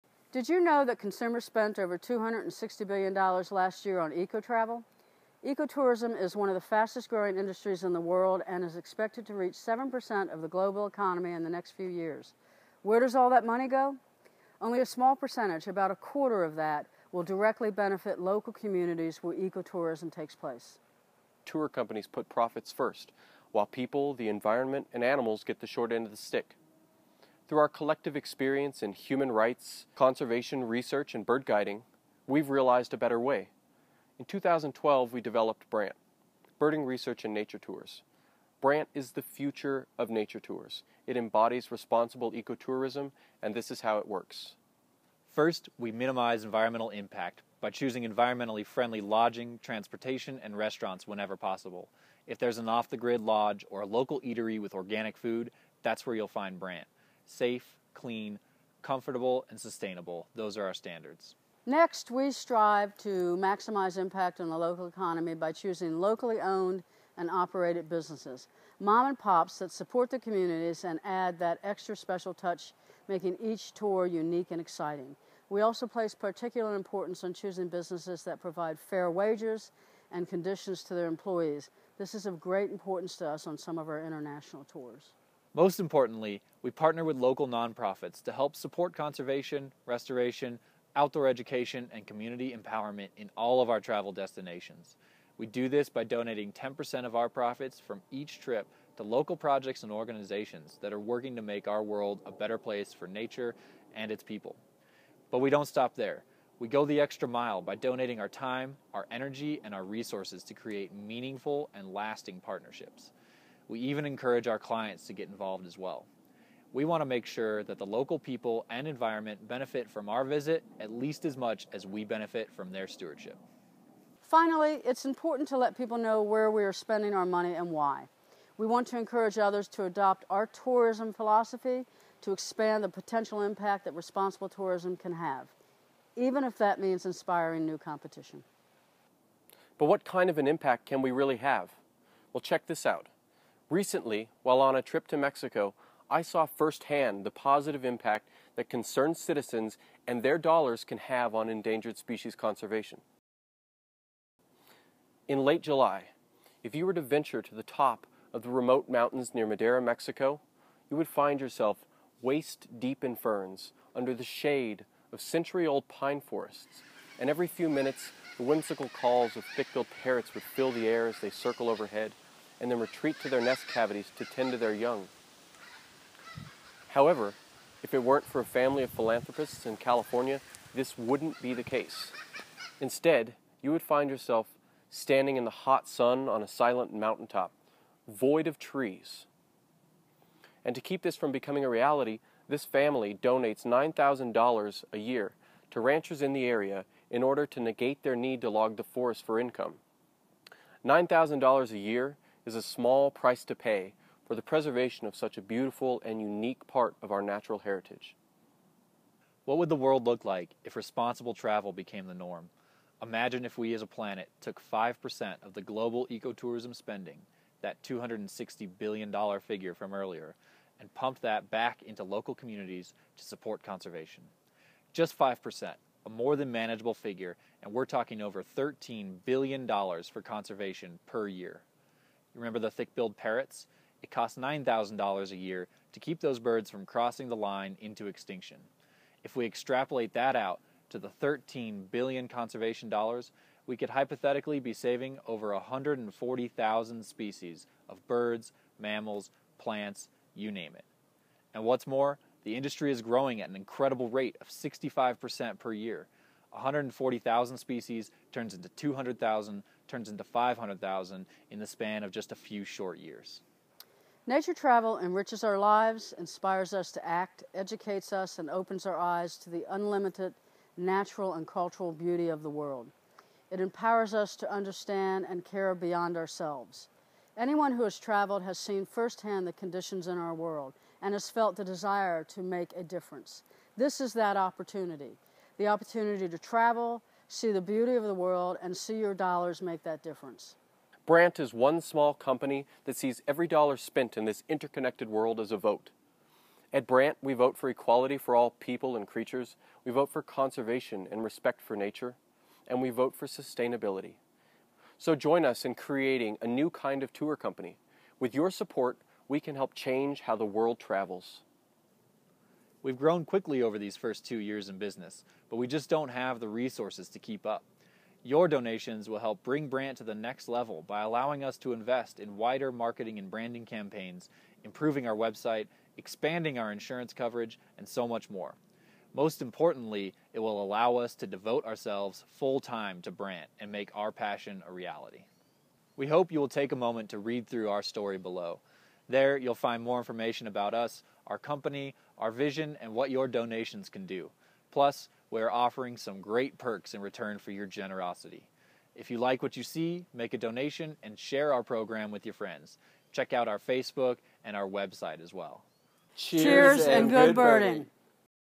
BRANT - Indiegogo Campaign
Born from a passion for birds, conservation and people, BRANT is an international nature travel company with a commitment to the environment through what we call Responsible Ecotourism. We created this company with the understanding that we would not only provide world-class nature tours but that the people and wildlife at our destinations would benefit from us as much as we benefit from them!